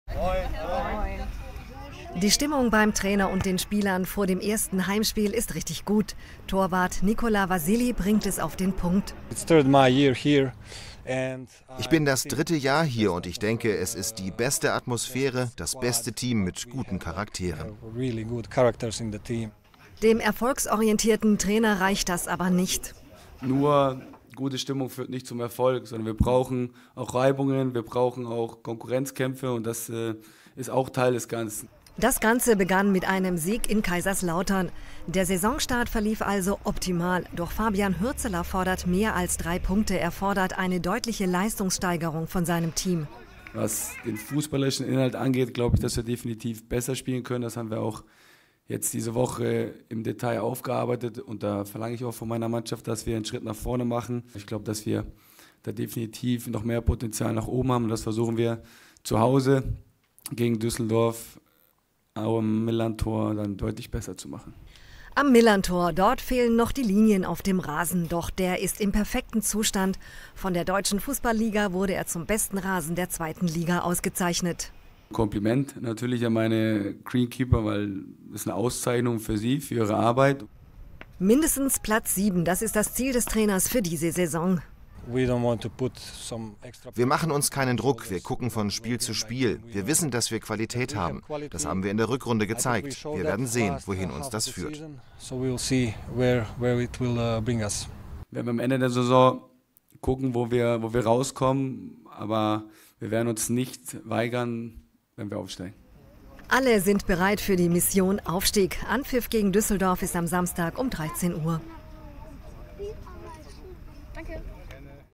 FC St. Pauli will auch gegen Düsseldorf drei Punkte holen
Der 2. Spieltag in der 2. Fußball-Bundesliga steht bevor. Am Samstag müssen die „Kiezkicker“ vom FC St. Pauli am Millerntor gegen Fortuna Düsseldorf ran. Beide Mannschaften gehen mit Siegen aus dem vergangenen Wochenende in die Partie. Während St. Pauli mit dem 2:1-Auswärtssieg in Kaiserslautern zurück nach Hamburg gefahren ist, hat die Fortuna zu Hause mit 1:0 die Hertha aus Berlin geschlagen. Nun kämpfen die beiden Mannschaften in der Hansestadt um drei Punkte. Anpfiff ist um 13 Uhr.